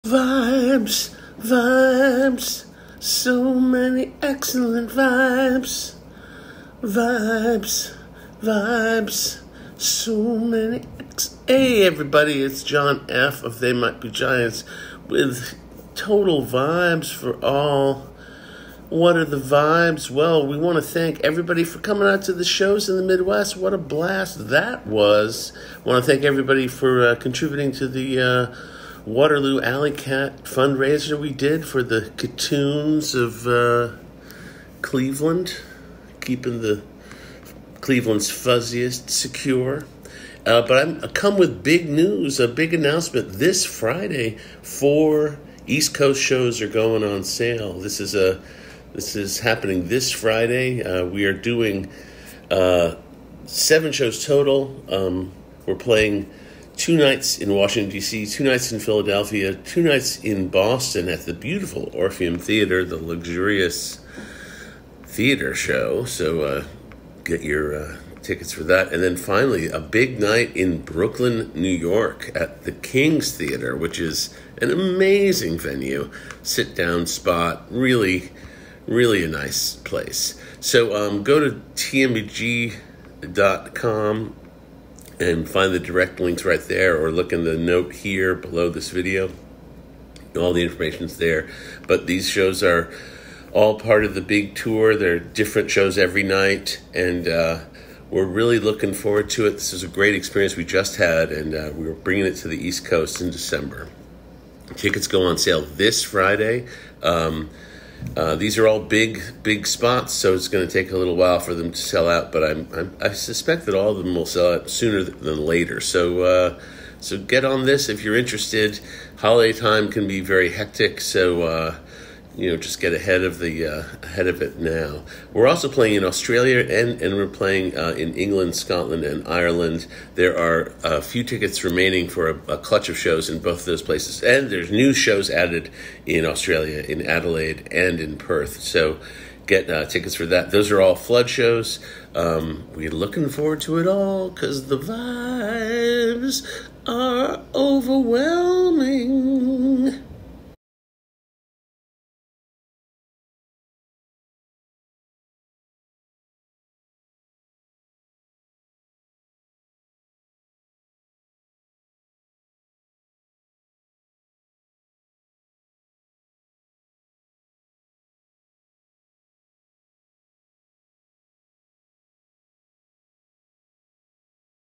TMBG announce East Coast run! DC BOSTON BROOKLYN PHILADELPHIA! On sale Friday!
A vibe report: 7 shows added. Big Brooklyn show at Kings Theatre.
On sale this Friday.

Philadelphia 12.5 and 12.6
Washington DC 12.7 and 12.8
Brooklyn 12.13 (Friday the 13th!)
Boston 12.14 and 12.15